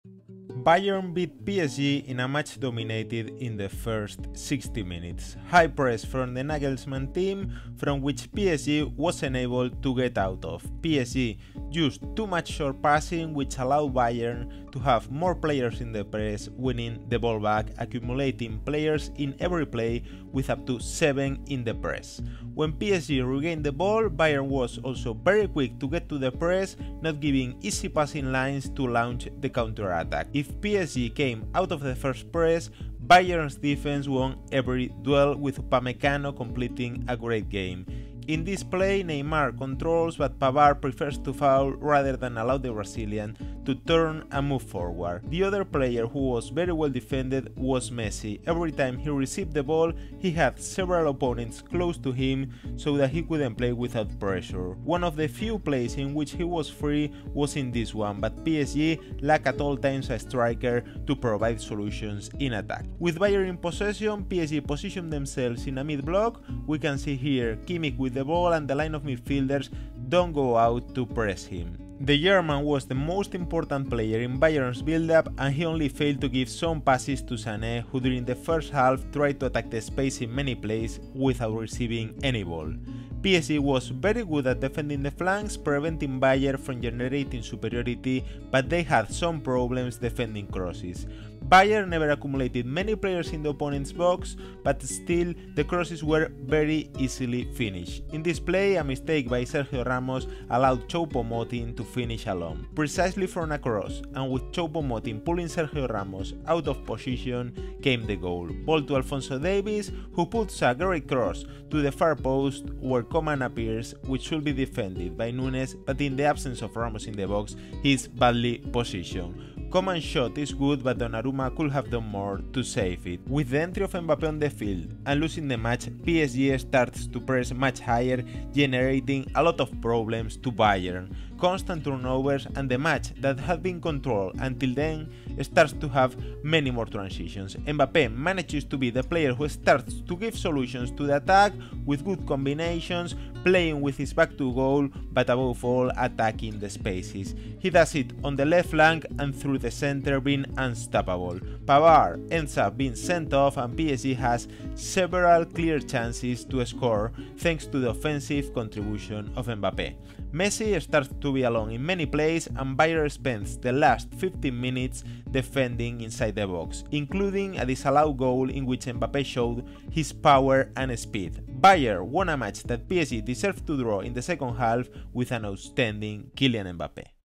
TACTICAL ANALYSIS: PSG 0-1 Bayern Munich - Mbappé vs Nagelsmann - Champions League
Analysis - PSG vs Bayern - Champions League